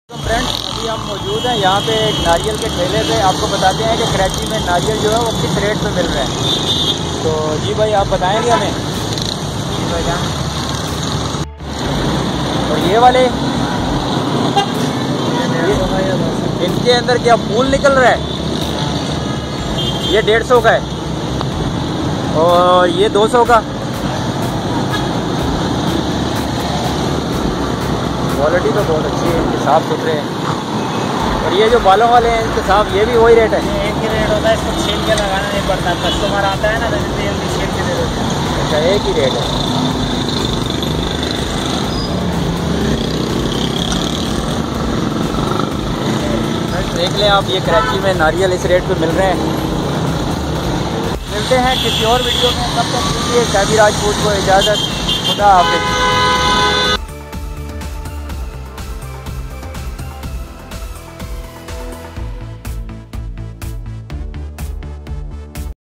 coconut price in karachi 2021
hi
i am saif rajput wellcome to uor youtube channel saifi vlogs
thanks
frienf main apko is video main karachi main coconut ki price bata raha hoon 2021 main kia price chal rehi he karachi city main.